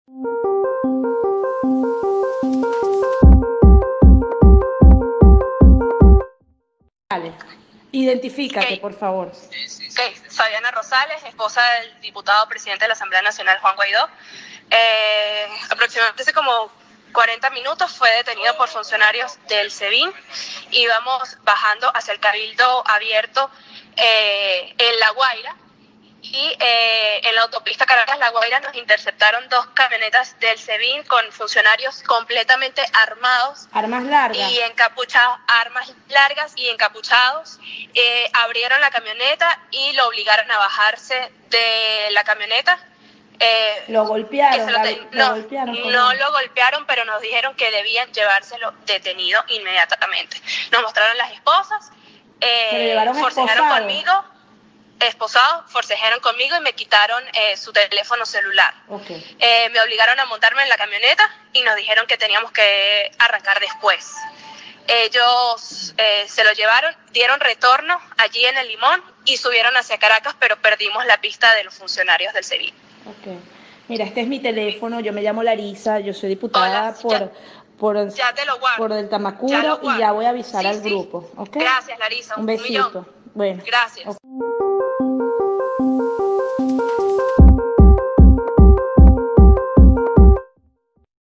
AUDIO| ESPOSA DEL PRESIDENTE JUAN GUAIDÓ
El presidente de la Asamblea Nacional –AN-, Juan Guaidó, fue detenido este domingo por el Servicio Bolivariano de Inteligencia Nacional –Sebin- cuando se dirigía a un cabildo abierto convocado por el parlamento  venezolano en Caraballeda, estado Vargas. Horas más tarde lo liberaron y anunció que iba camino a Caraballeda, donde se realizaría el Cabildo Abierto convocado para este domingo.

Fabiana Rosales, esposa del presidente de la Asamblea Nacional, informó que Guaidó fue obligado a bajarse del vehículo por funcionarios armados y encapuchados del Sebin. Rosales, quien acompañaba a su  marido, precisó que fue esposado y despojado de su teléfono celular.

“Nos dijeron que debían llevarselo detenido inmediatamente“.